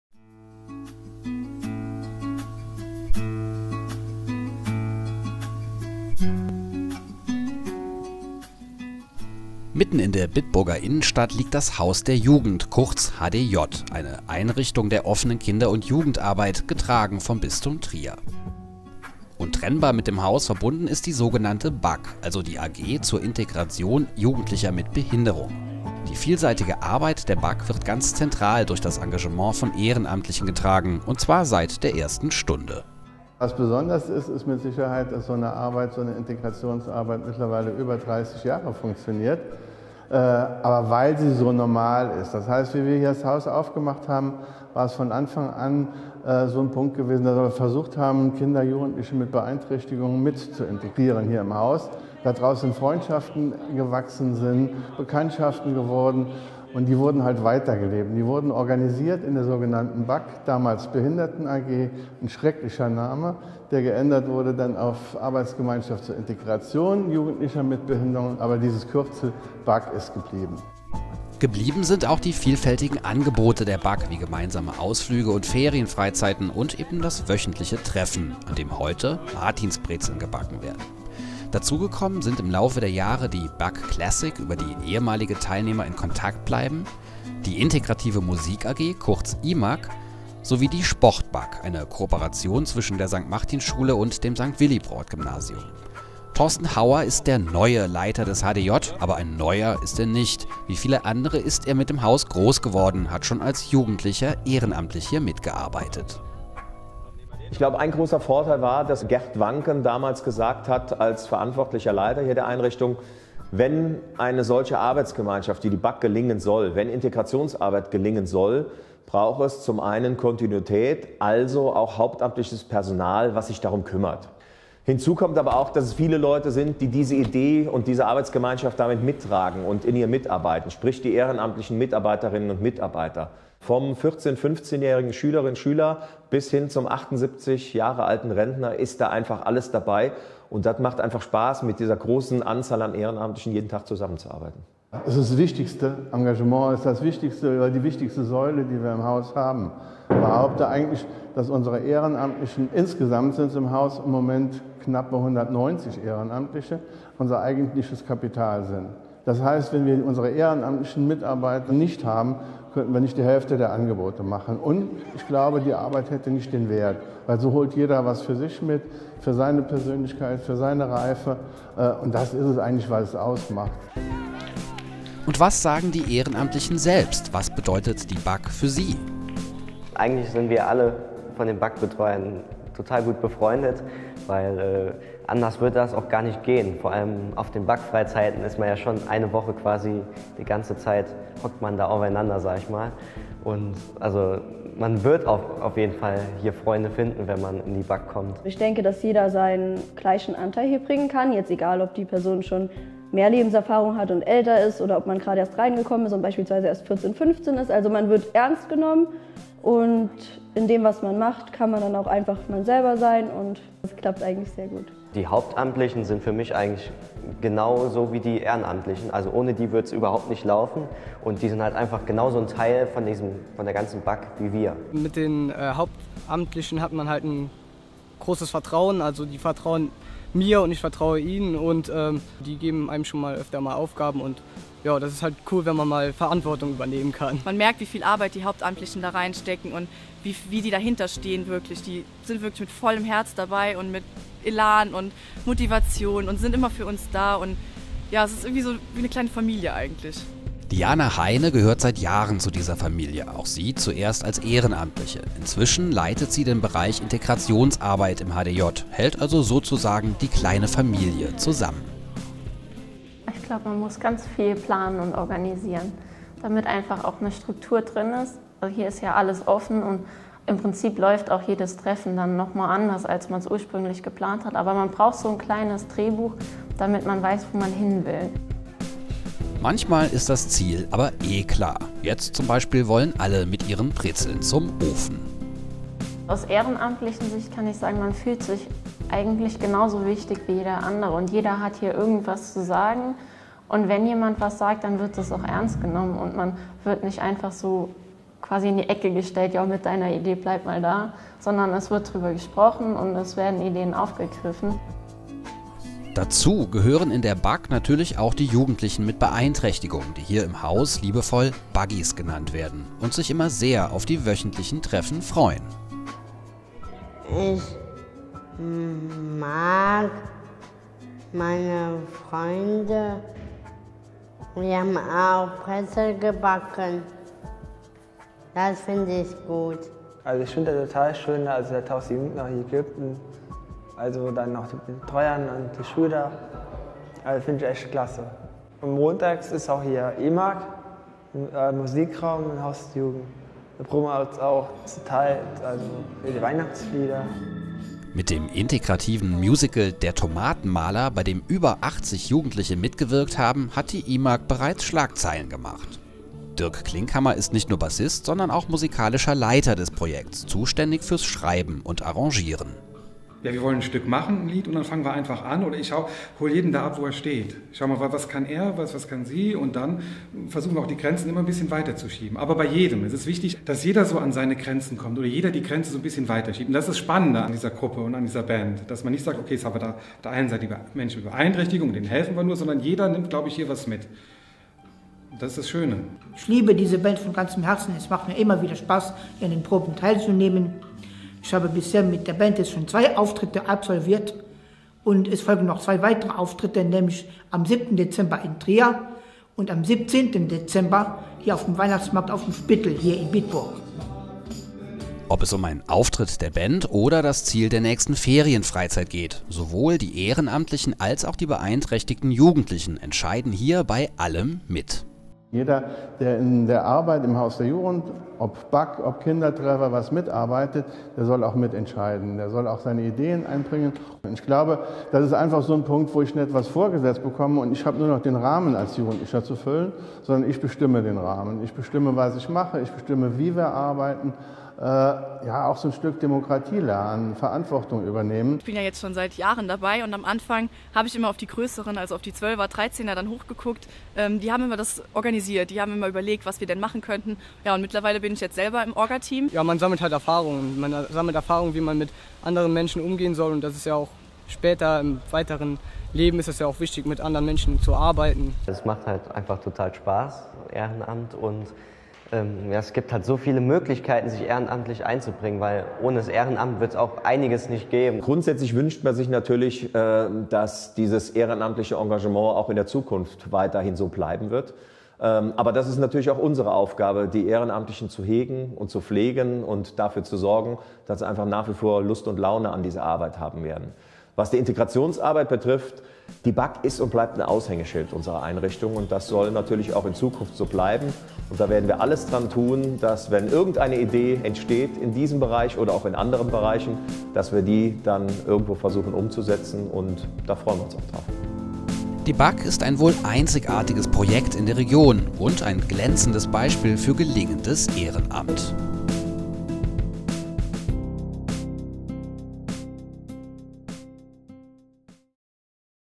Integration im Bitburger Haus der Jugend - Glänzendes Beispiel für gelungenes Ehrenamt
Im Bitburger Haus der Jugend (HDJ) findet seit mehr als 30 Jahren Integrationsarbeit mit Kindern und Jugendlichen mit Beeinträchtigungen statt. Die Arbeit dort wird vor allem von Ehrenamtlichen getragen. "Das ist es, was die Arbeit ausmacht", sagt Gründer Gerd Wanken.